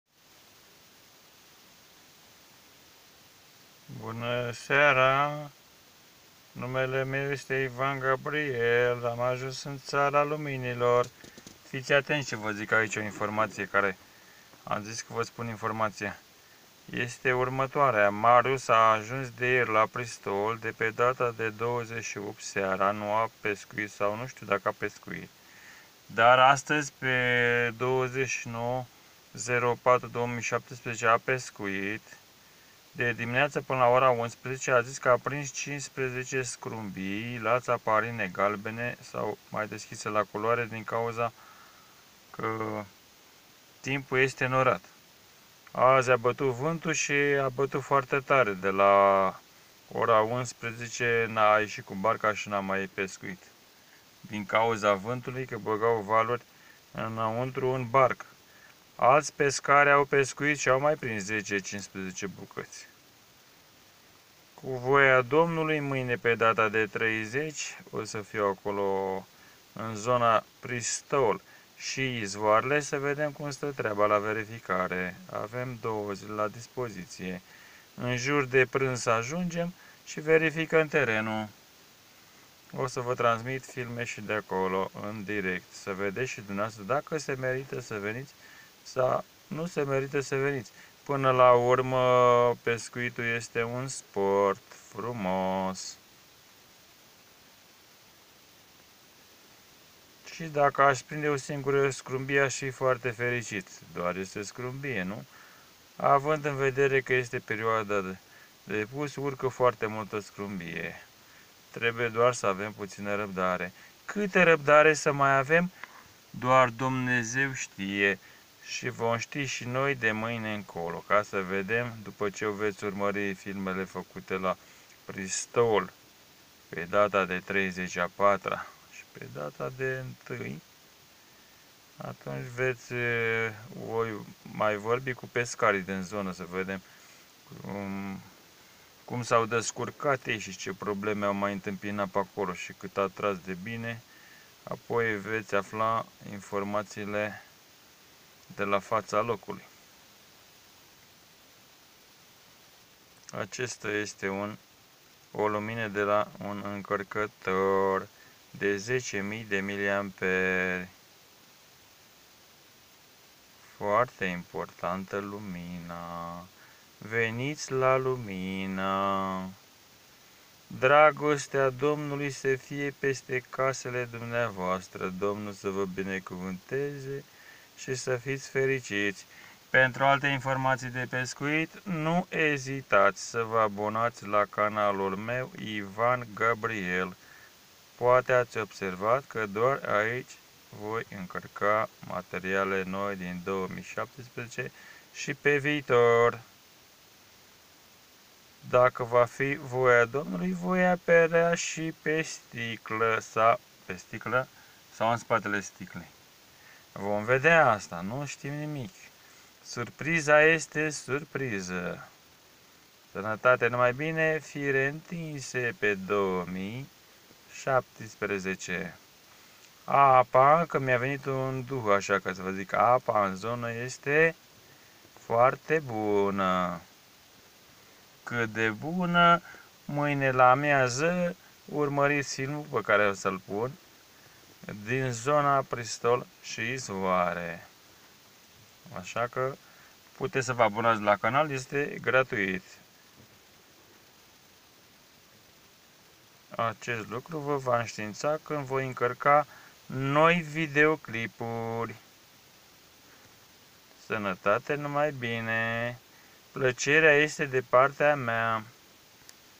Informații pescuit scrumbie 29_04_2017
Scrumbia s_,a prins la țaparine galbene la Izvoarele Mehedinti